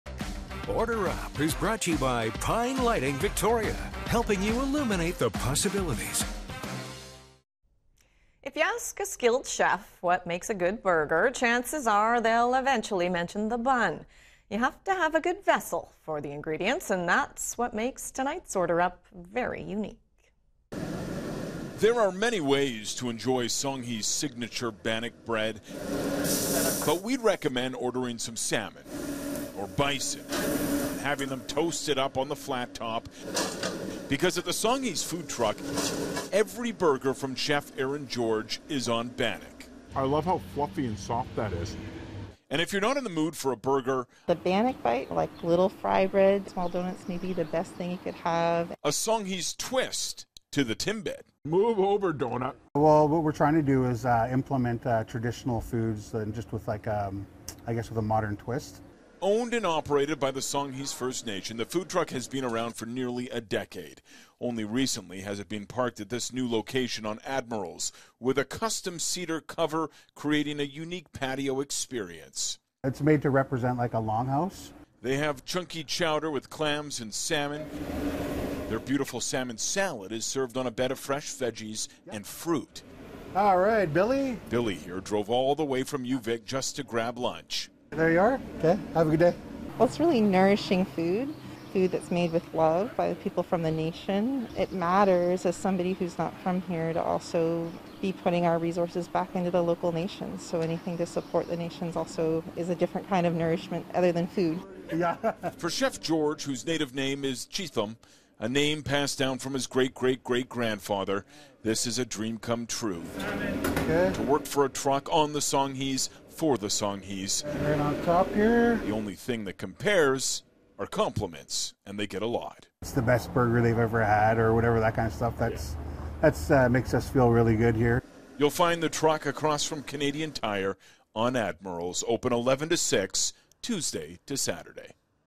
Order Up: Songhees Food Truck
It's home to some delicious bannock served in a variety of ways: burgers on bannock, bannock balls full of sugar, or straight up bannock with some delicious homemade jam. The Songhees Food Truck has it all and a whole lot more.

The Songhees Food Truck is located at 1502 Admirals Road